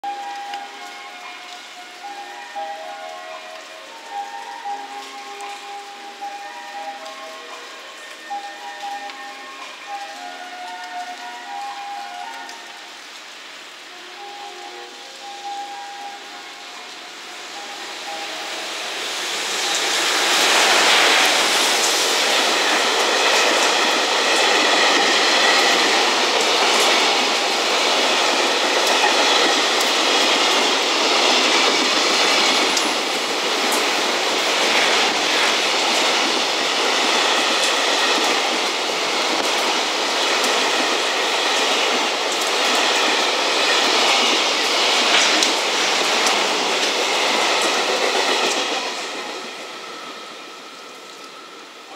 山陽本線 下り 貨物列車 69レ EF210-111号機［新］ 牽引 コンテナ 24B JR貨物 新鶴見機関区 横川 2020.6.11 01845
山陽本線 下り 貨物列車 69レ EF210-111号機［新］ 牽引 コンテナ 24B JR貨物 新鶴見機関区 横川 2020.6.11